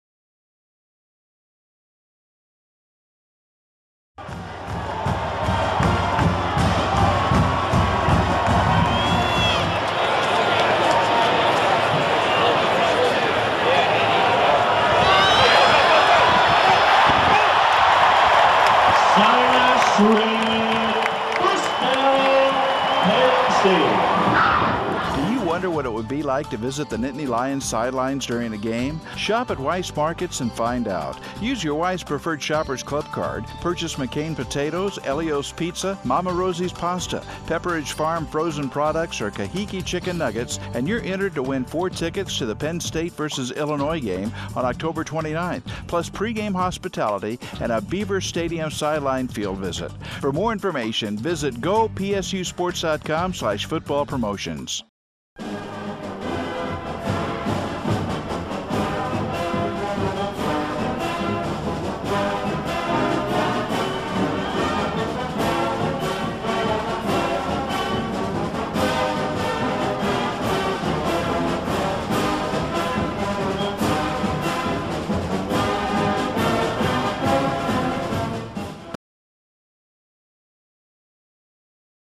2011 ACOSTA SALES AND MARKETING PENN STATE FOOTBALL RETAIL PROMOTION
This piece includes the marketing elements used for the Acosta Sales and Marketing promotion featuring Mama Rosie's Pasta, Kahiki Chicken, Pepperidge Farm Frozen products, McCain Potatoes and Ellio's Pizza. Fans were encouraged to shop at Weis Markets in the month of October using their Weis Preferred Shoppers Club Card and win the VIP treatment for the Penn State-Illinois game. The winner had a great time as Penn State won the game 10-7 giving Joe Paterno his 409th win.